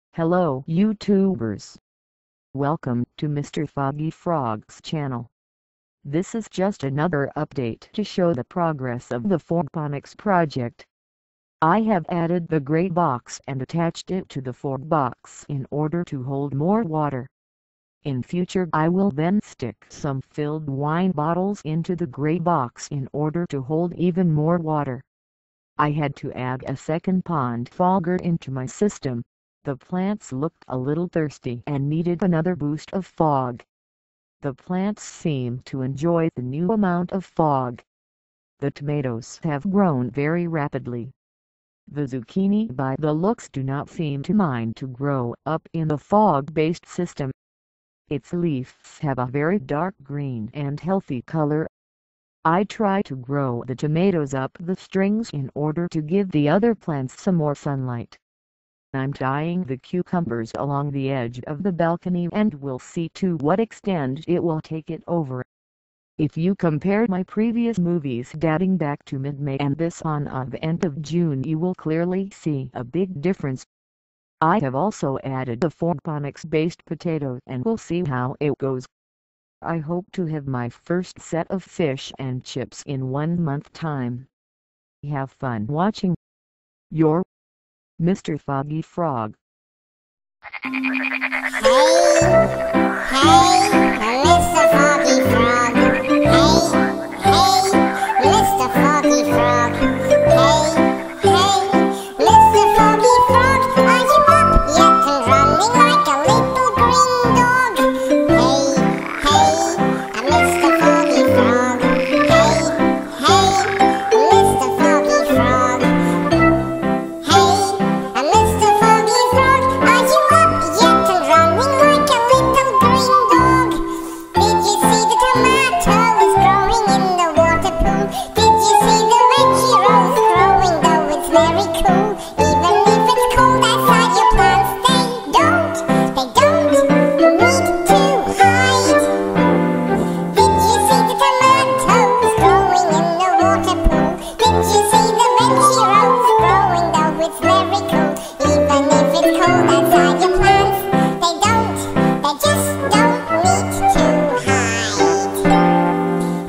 2012.06.26 Fogponics update - MrFoggyFrog
I have added another update of the running fogponics system. The plants are growing at a steady rate. Just to give you guys the opportunity to follow the progress.

Your

MrFoggyfrog